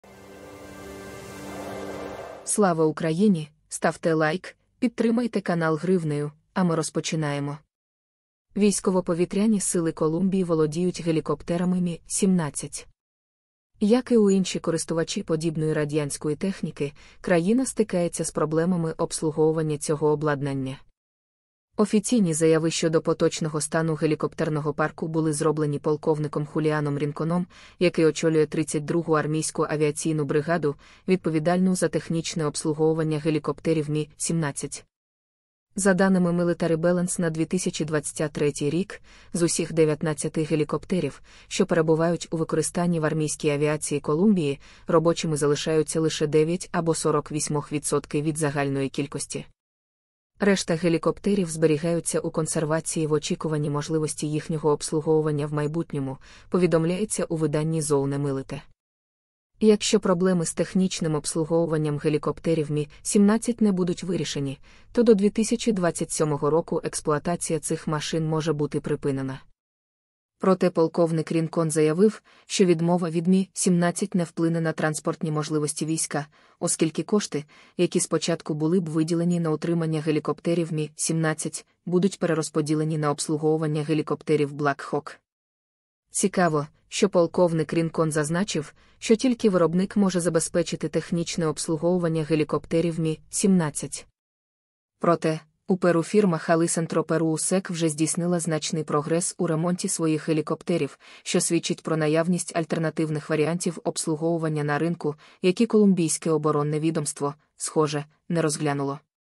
Колумбія могла передати свої Ми-17 Україні
USDT: TSJfcHW5UBth2qsR6jYUeGsxJDYYL16YCL

Bitcoin
BTC: 1PYFjXGCBvzyjpbuPRiy1P7DwndWNcTKfG